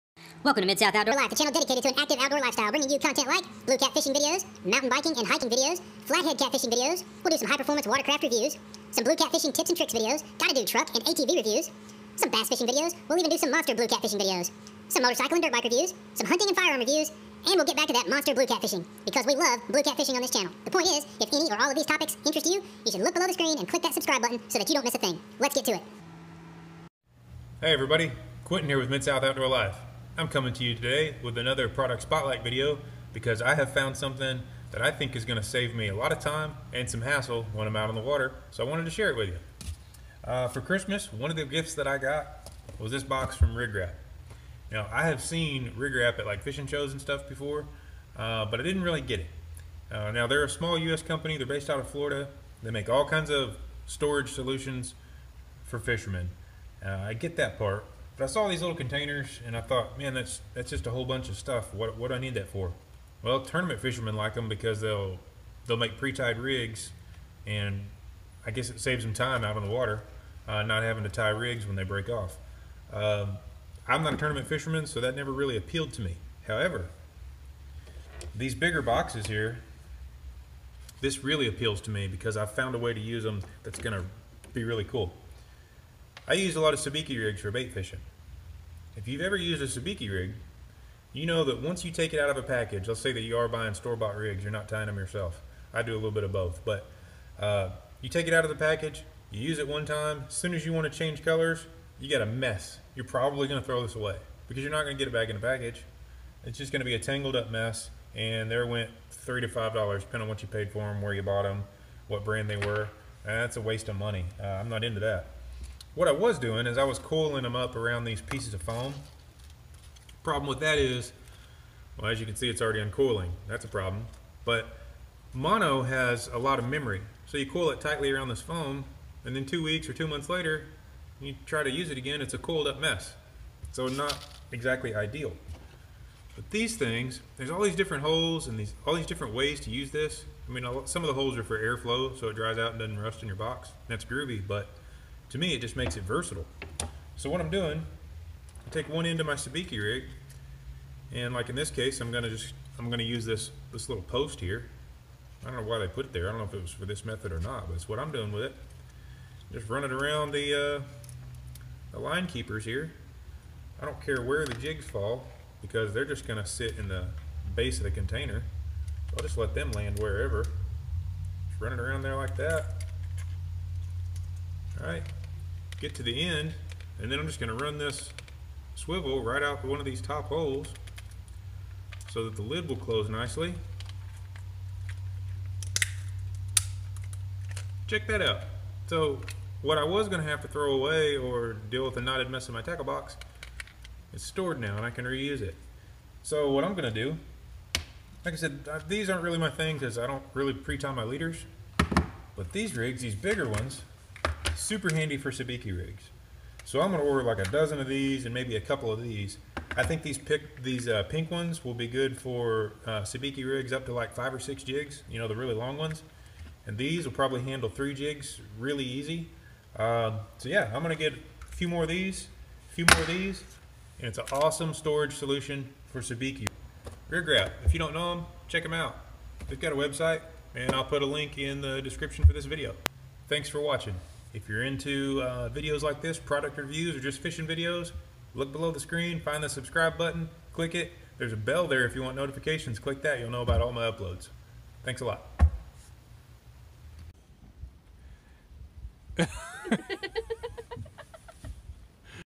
EASY SABIKI RIG STORAGE (how to store and save Sabiki rigs)
Sabiki rigs are used to catch bait fish all over planet Earth. In this video I show an easy storage solution for Sabiki rigs. Stop throwing away Sabiki Rigs and start saving money.